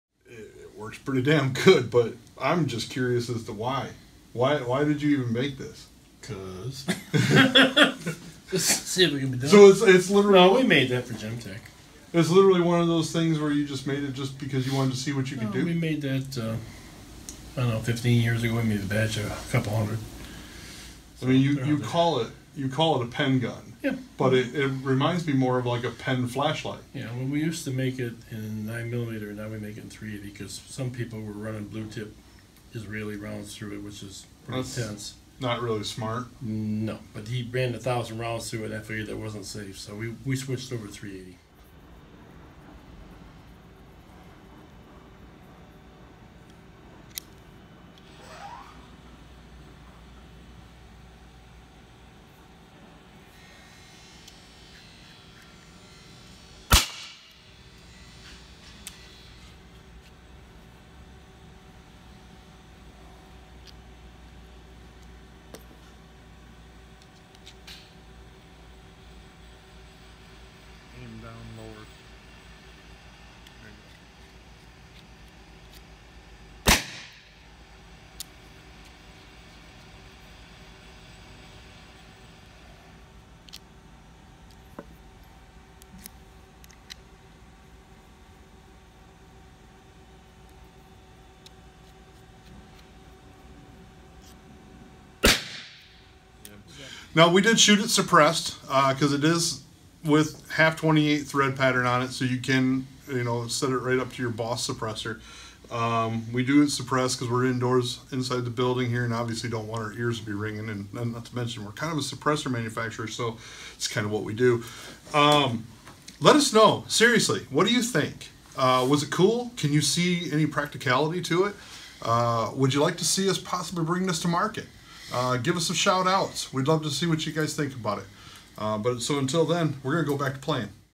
The GSL Supressed .380 Pen Gun
Introducing the experimental GSL .380 Pen Gun. Originally designed for another company within the suppressor industry, the GSL Pen Gun offers a last-ditch option in self-defense and can be made hearing safe with GSL's Boss 9mm Suppressor. Greg would like to know your thoughts. Would you like to see the GSL Pen Gun in our product line this year? Learn more about the GSL 9mm Boss Suppressor